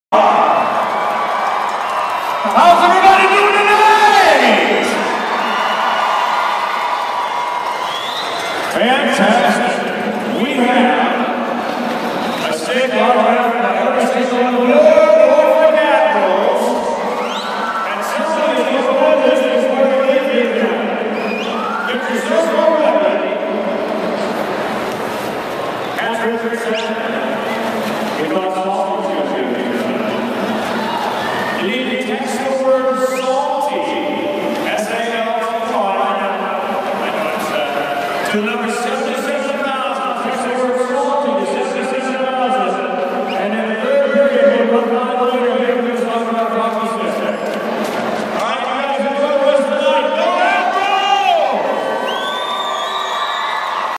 Rumble on Ice
Rick on the Ice during the first intermission of the Admirals 2-1 win Saturday Night March 26th 2011 against the Manchester Monarchs